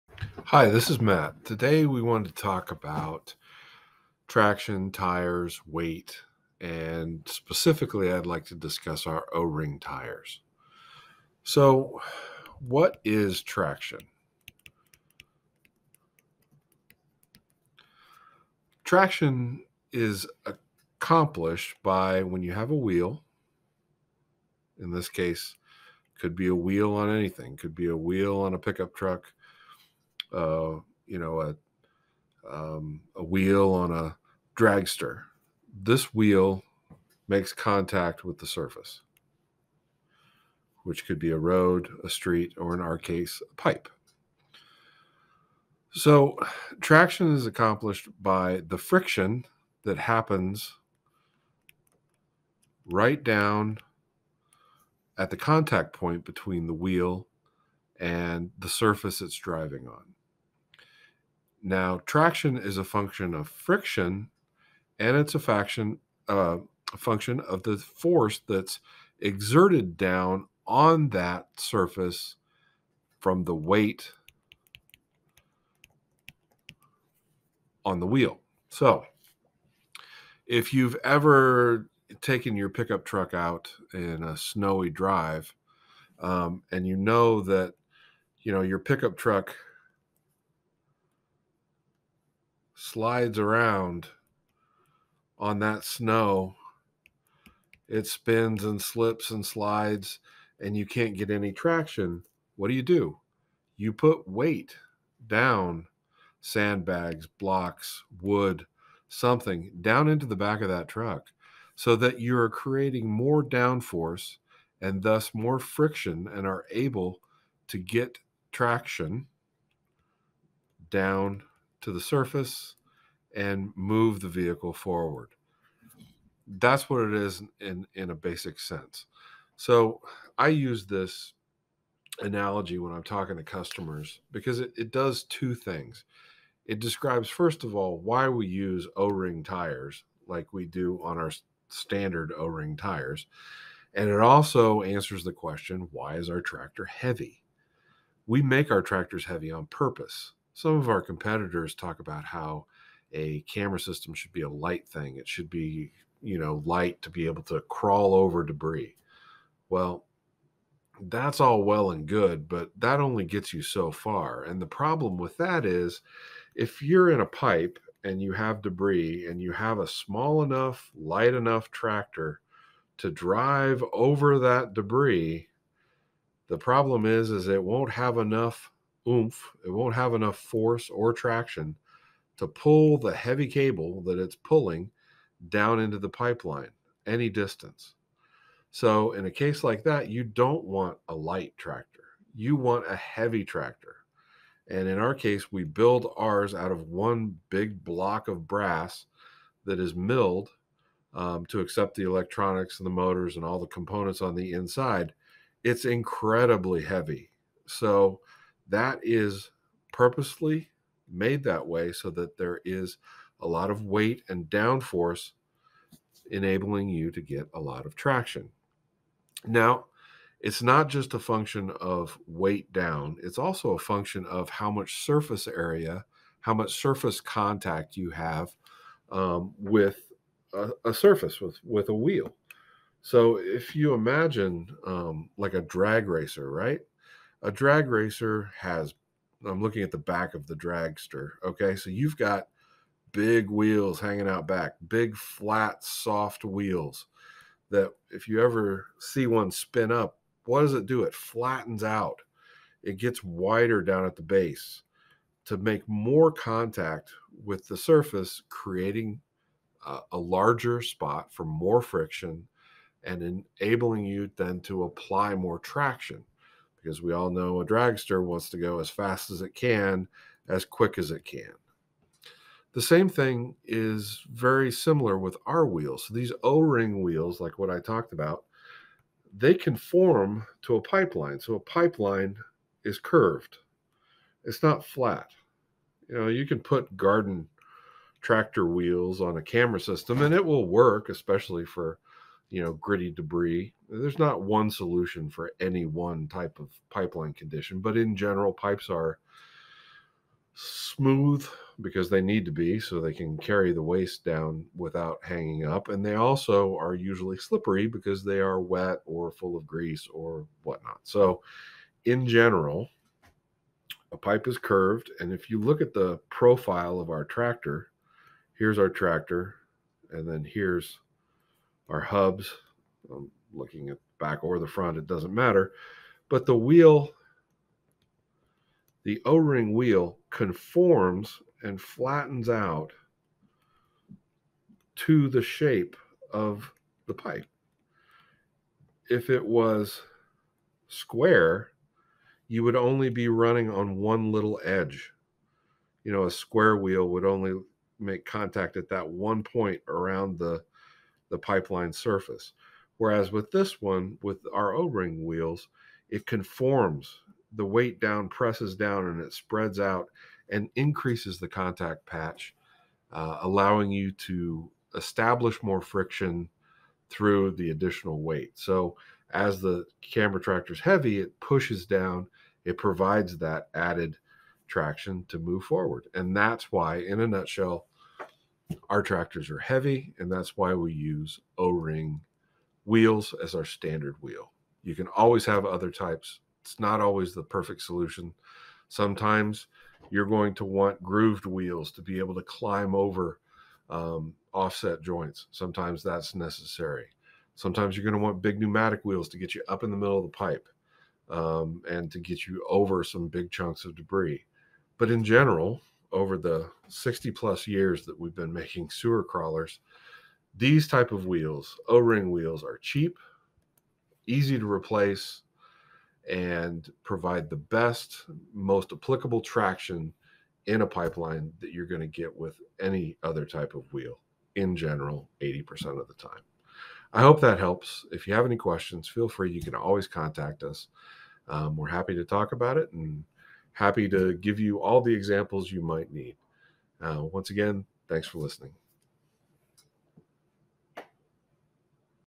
Traction and Standard Wheels
IBAK systems are capable of pulling longer distances and through slippery lines because they are built out of dense materials that provide the maximum traction.  In this video, Matt Sutton, VP of Sales and Marketing at RapidView, explains this concept and it's advantages.